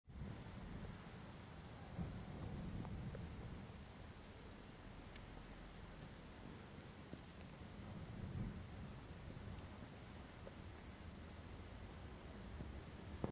Incendio Serra d'El-Rei 2017.07.20 18:05 (2)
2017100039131 Estado Ocorrência LEIRIA PENICHE Serra d'El-Rei SERRA DE EL REI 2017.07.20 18:05 Povoamento Florestal 48 Operacionais 12 Meios terrestes 0